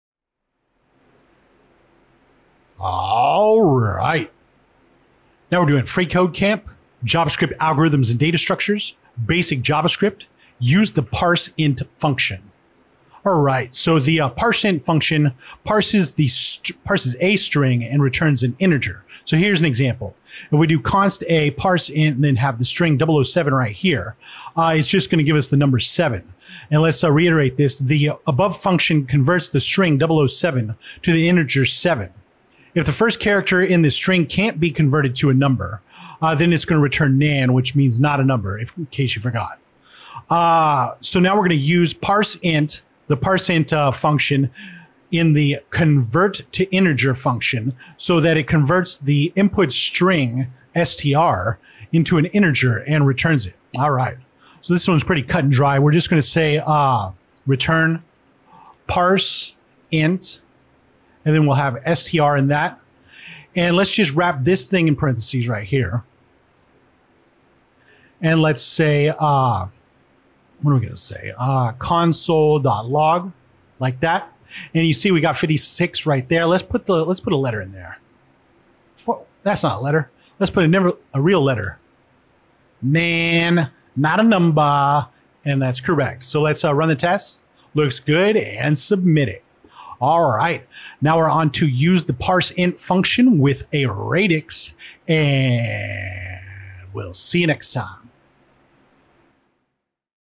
JavaScript Basic 108: Use the parseInt Function | FreeCodeCamp
🌟 **"Deciphering Digital Mysteries: The Power of `parseInt()` Revealed!"** 📜✨

Greetings, Digital Detectives! 🔍👓

Has there ever been a time when a string of numbers, veiled in text form, mystified you, leaving you yearning to transform it into a tangible number? Say no more! Embark on an enchanting escapade to unveil the power of the `parseInt()` function!

🎬 **Today's Enchanting Expedition**:

- **From Text to Truth**: Dive deep into the heart of `parseInt()`, a function  crafted by coders of yore to turn textual digits into actual numbers! 📜🔍
- **Unraveling Secrets**: Discover how `"007"` is not just a code but can leap off the parchment to become the integer `7` in a mere flash! 🎩✨
- **Guarding Against Mysteries**: Learn how, if faced with an inscrutable text, our guardian `parseInt()` responds with the riddle of `NaN`. 🌌🛡️
- **Craft Your Own Codex**: Join us in shaping the `convertToInteger` spell, harnessing the power of `parseInt()` to convert any enigma, any string `str`, into its numeric essence. 🎨📖

Join our caravan, traverse the realms of numbers and text, and unlock the secrets of JavaScript's alchemical wonders! By journey's end, you'll be not just a coder but a sorcerer, wielding `parseInt()` to reveal truth from the most cryptic of textual riddles!